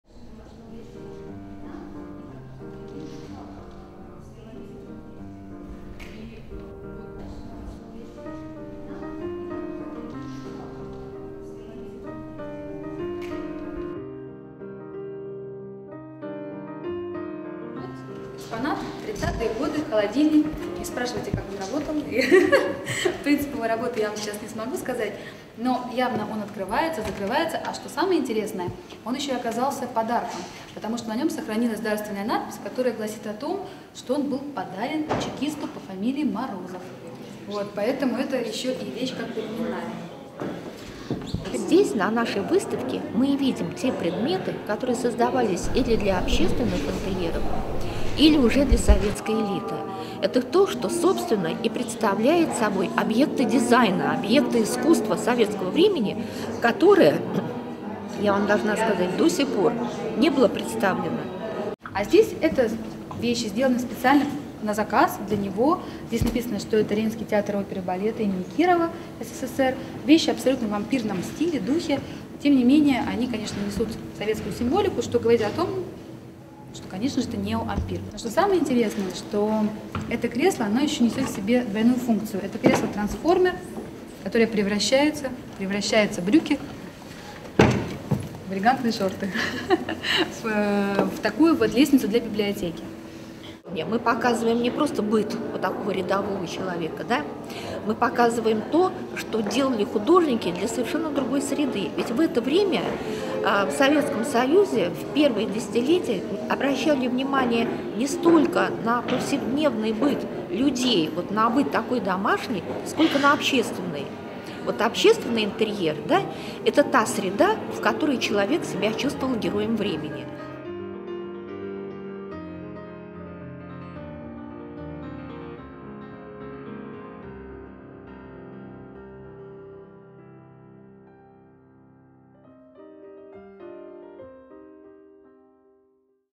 Exposition sur le design soviétique à Moscou
Jusqu’au 22 mars, le Musée d’architecture Chtchoussiev de Moscou propose à ses visiteurs de découvrir l'exposition « Design soviétique. Du constructivisme au modernisme. Années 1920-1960 ».